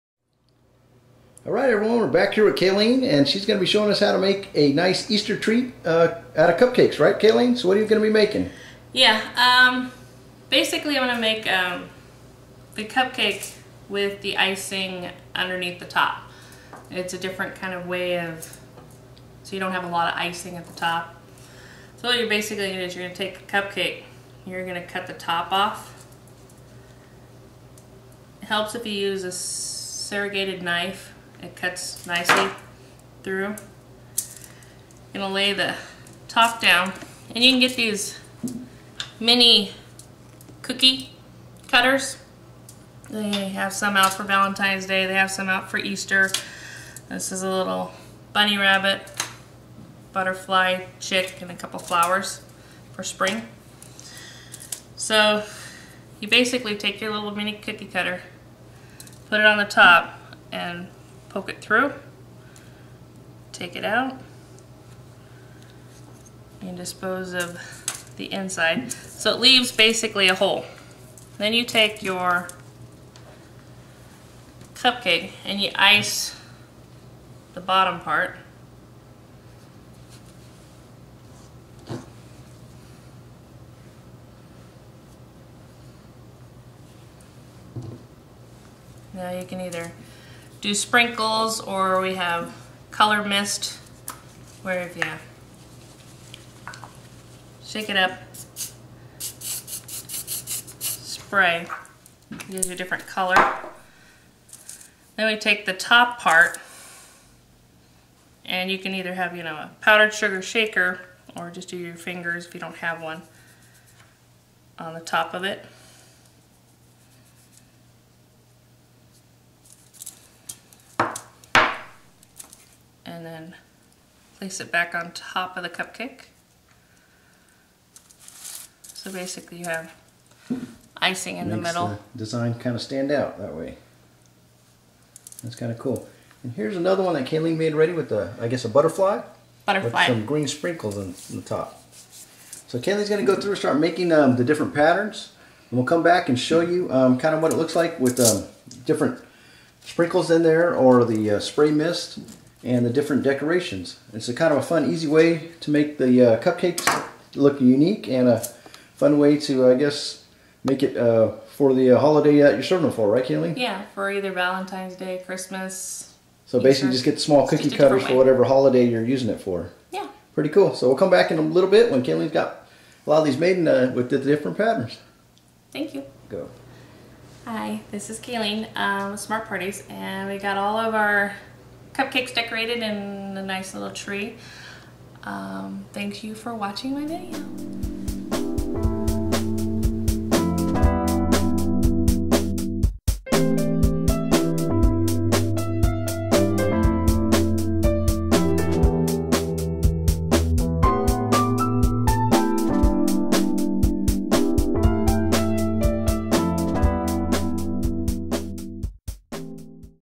EasterCupcakes.wmv
A new and diffrent way of decorating cupcakes for easter.